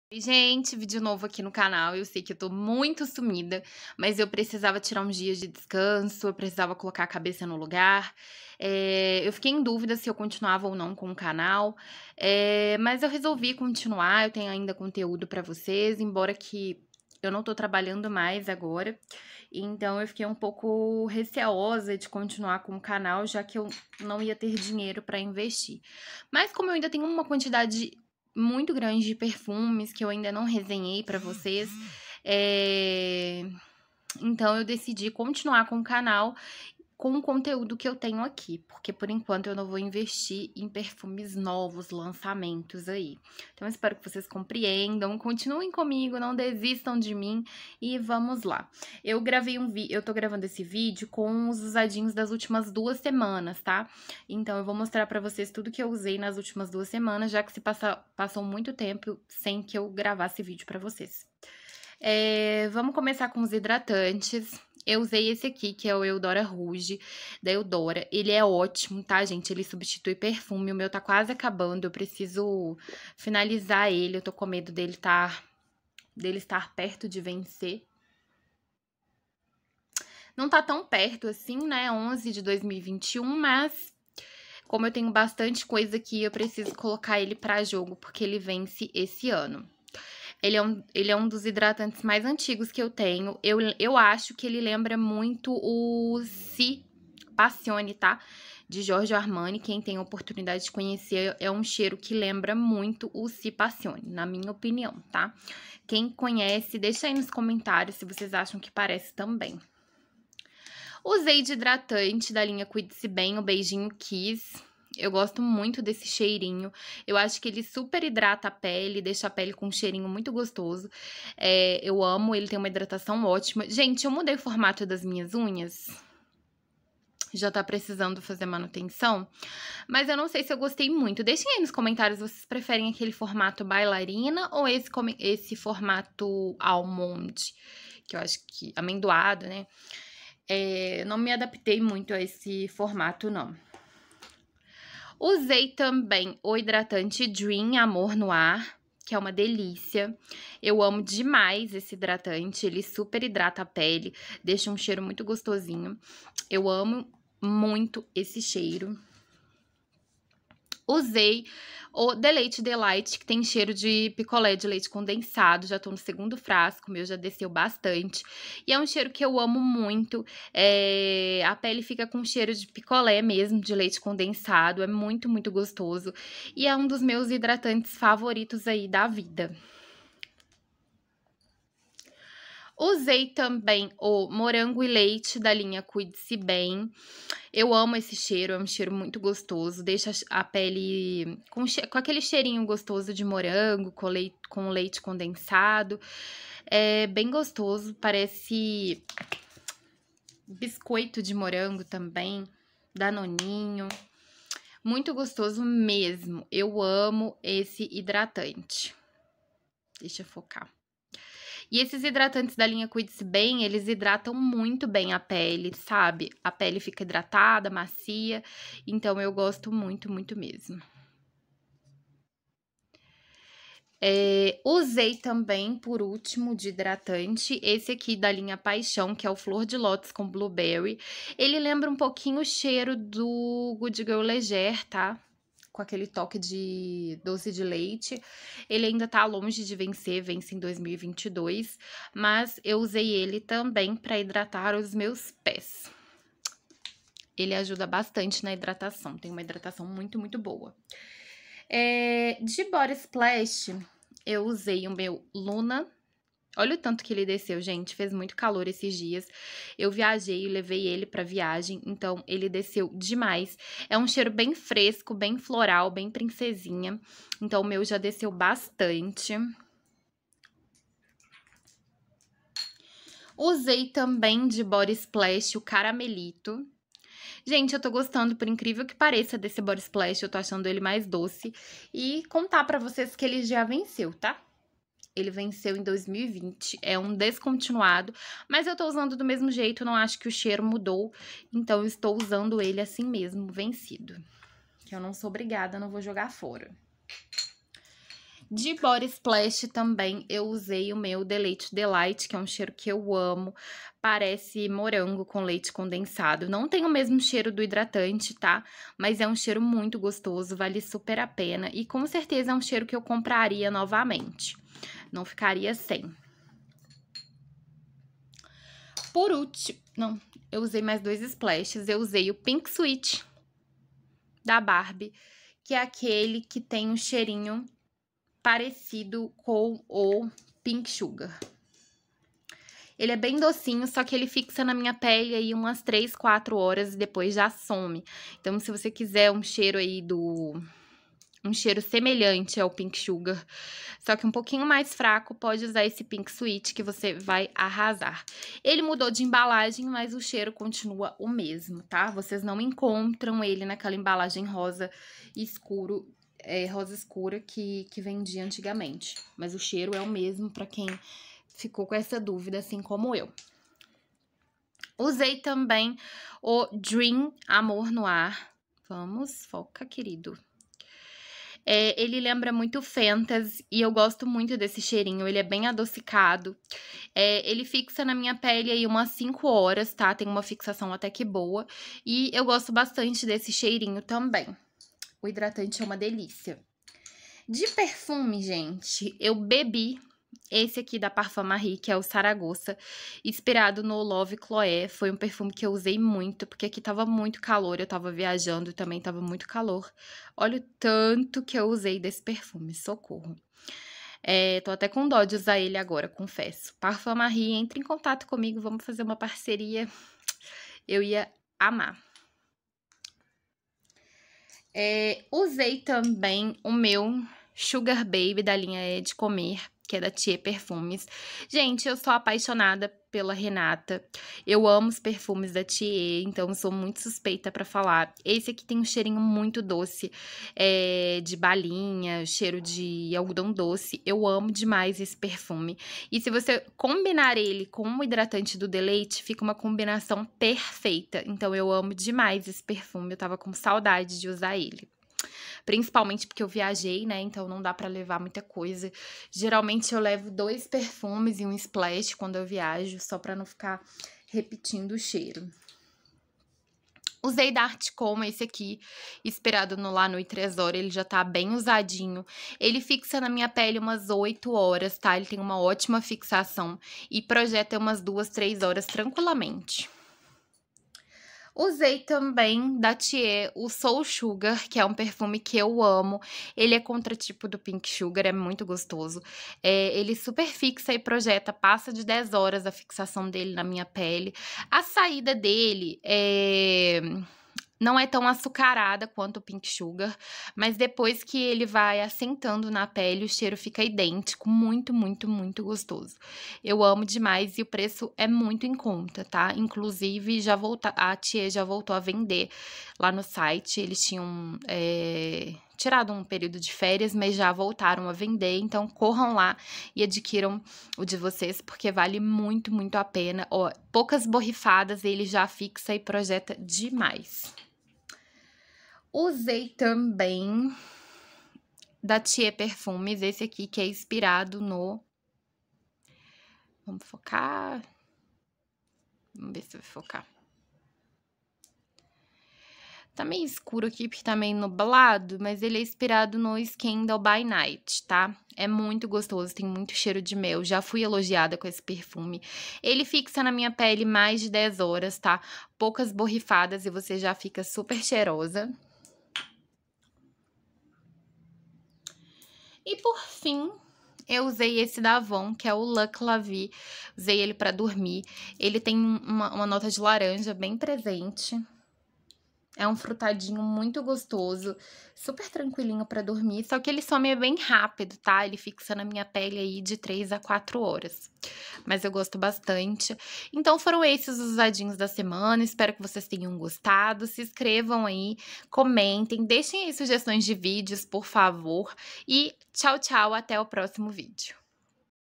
USADINHOS DA ÚLTIMA SEMANA ❤️ - POR QUE SUMI? ☀️🌸
Oi Gente, nesse vídeo mostro pra vocês meus usadinhos da semana! 🌼🌻

Parceria do canal: Dani Cavalcanti

Meu Instagram 🧡: @amorporperfumes